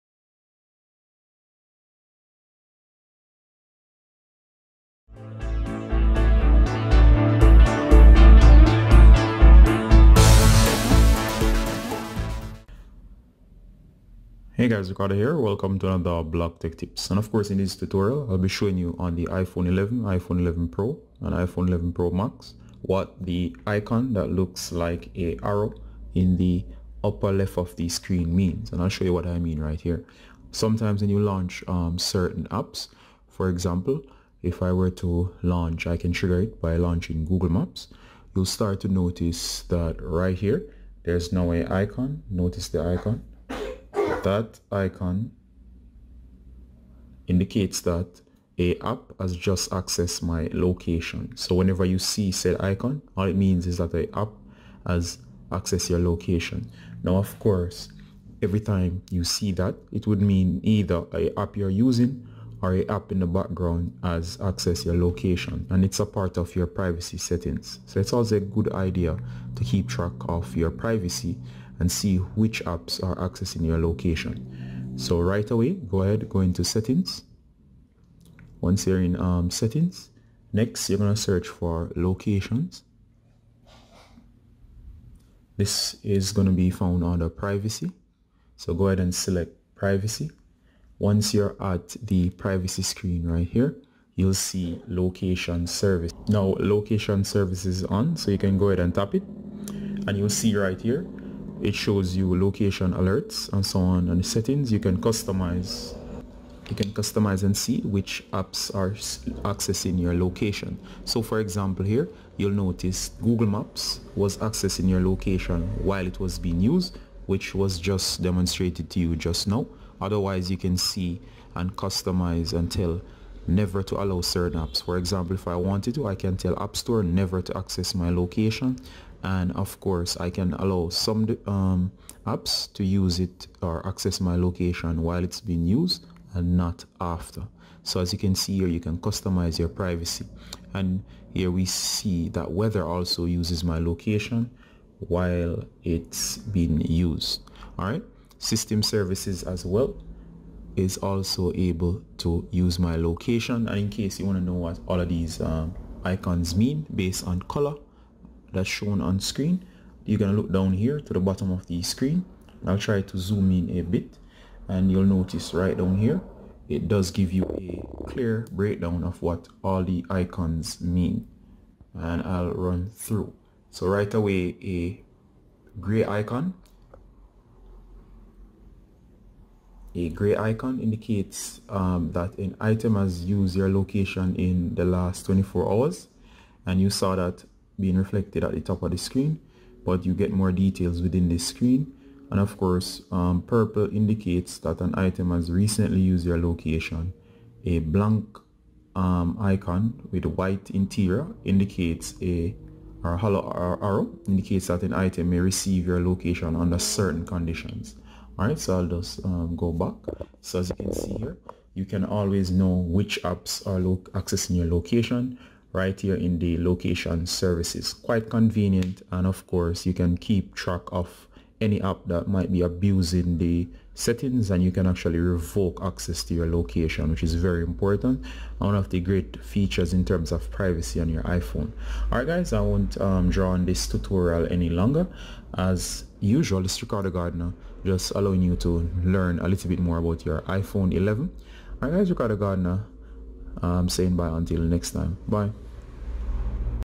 iPhone 11 Location icon
Find out what the iPhone 11 Location icons mean and which apps is accessing your location and control access for your privacy right now. Works for the iPhone 11 Pro and the iPhone 11 Pro max as well.